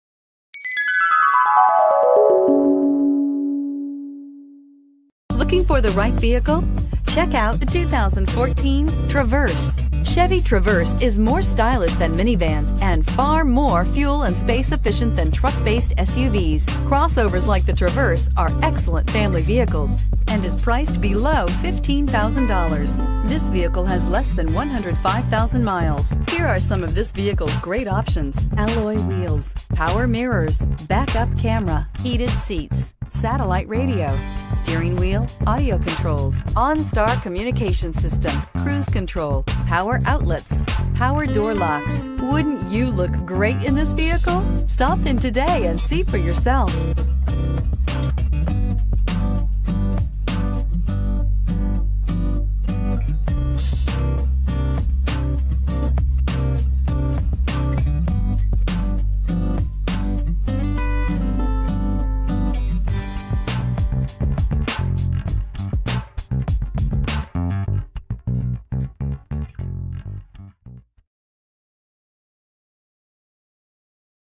2014 Chevrolet Traverse St. Peters MO 25787-1
2014 Chevrolet Traverse LT w/1LT

Armed with a remote starter, backup camera, heated seats, Sirius satellite radio, OnStar Communication System, side air bag system, and digital display, you won't be able to get enough of this 2014 Chevrolet Traverse LT w/1LT.  It comes with a 6 Cylinder engine.  This one's a deal at $14,277.  You can trust this SUV AWD because it has a crash test rating of 5 out of 5 stars!  The exterior is a ravishing white diamond tricoat.  Stay focused while driving with the Bluetooth feature.  You won't miss a beat with great time-saving features like remote starter.  Music-lovers won't want to get off the road with great audio features CD player.  Interested? Call today to take this vehicle for a spin!